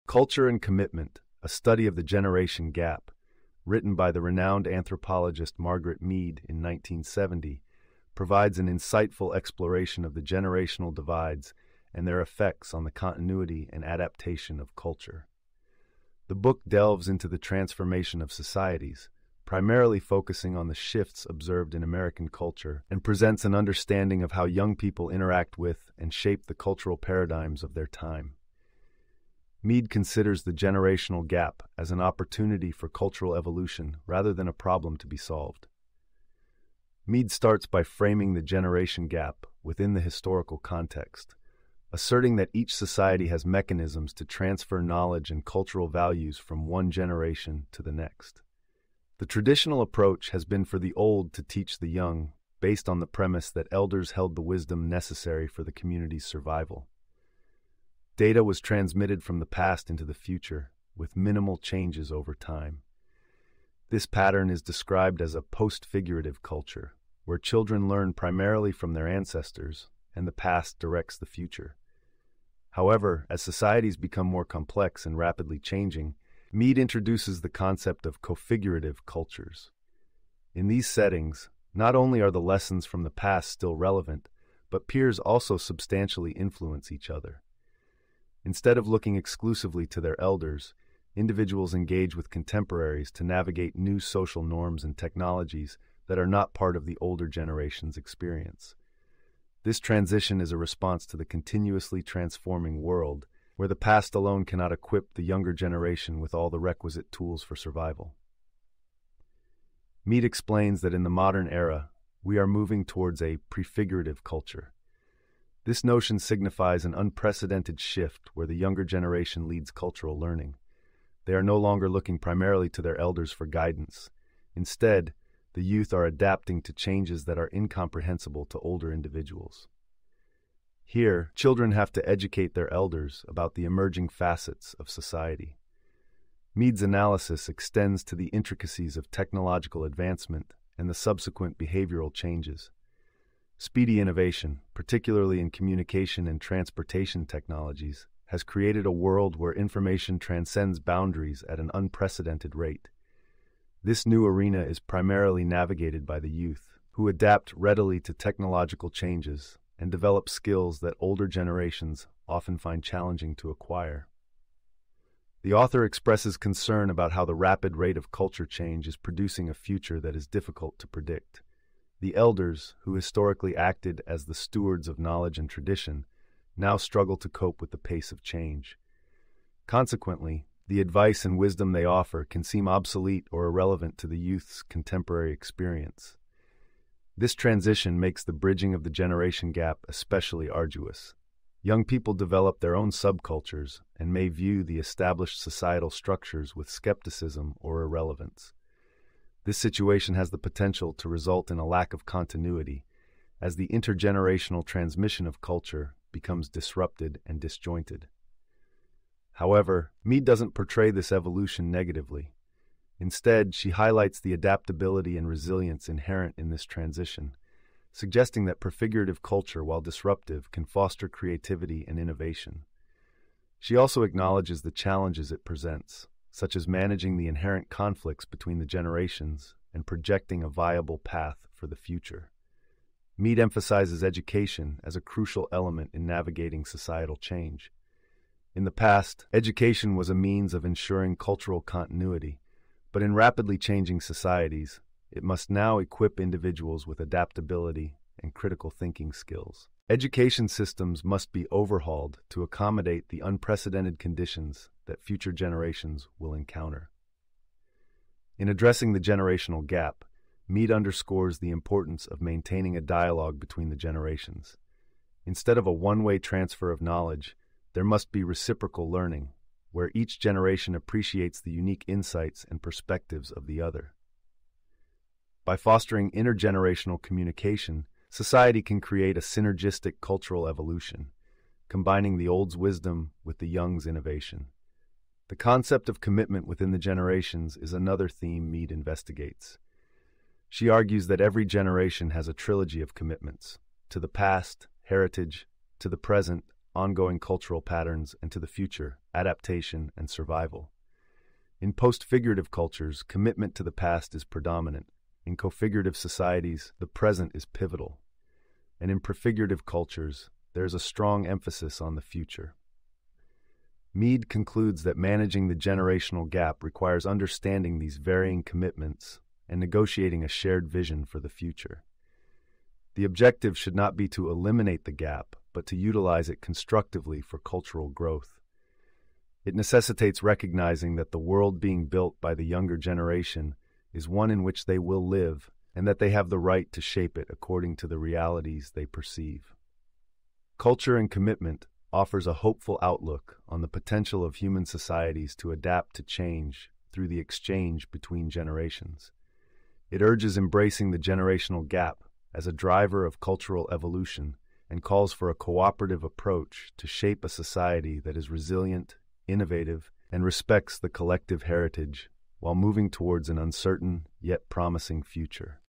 Margaret Mead - Culture and Commitment, A Study of the Generation Gap (1970)
"Culture and Commitment: A Study of the Generation Gap" by Margaret Mead explores the dynamics between generations, particularly how rapidly changing societies create gaps in understanding and values between the young and old, necessitating new forms of transmission of culture and commitment across age groups.